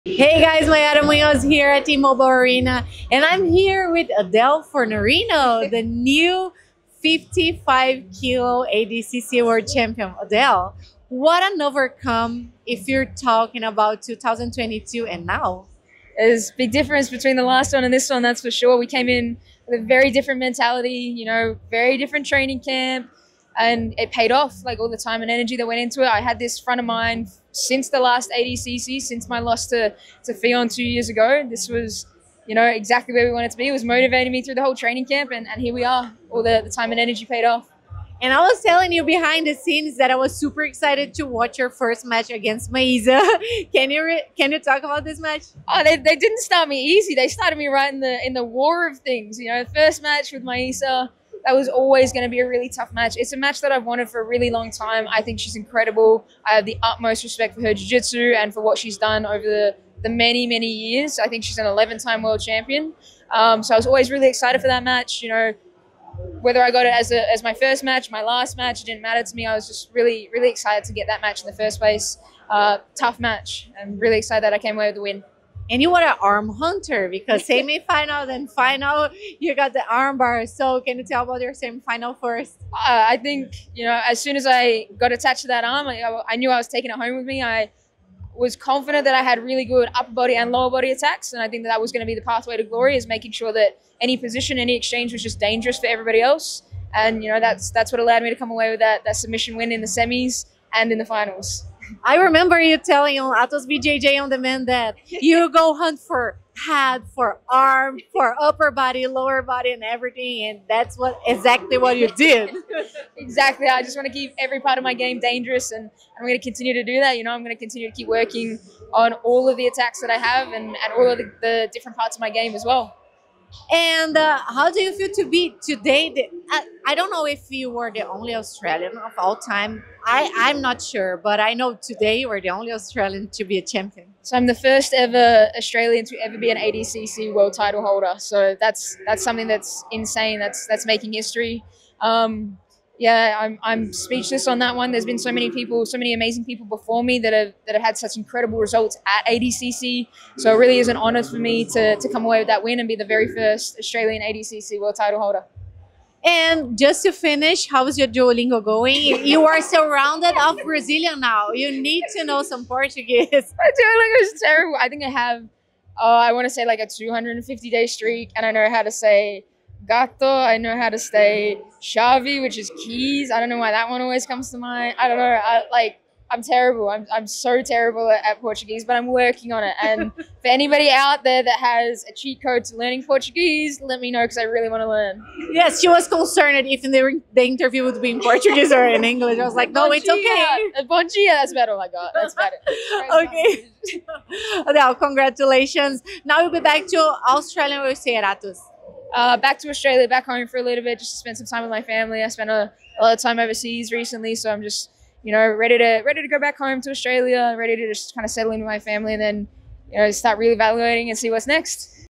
Adele Fornarino tornou-se a primeira australiana a vencer um título do ADCC
Adele Fornarino foi campeã na categoria até 55kg. A entrevista foi antes do ouro duplo, mas ela ainda voltou e venceu o absoluto!

Nosso podcast está aqui nesse link e você pode escolher seu lugar favorito para ouvir